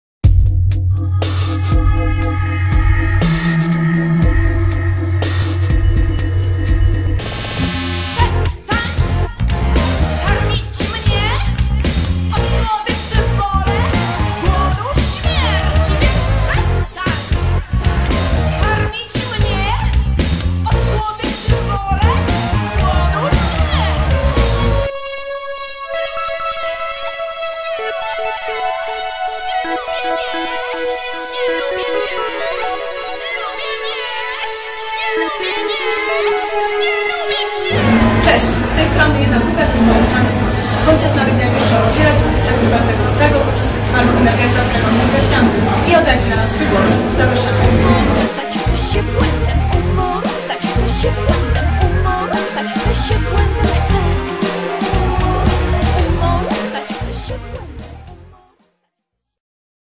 Stan Snu teaser 2013
teaser przed koncertem na eliminacje do Emergenza Festival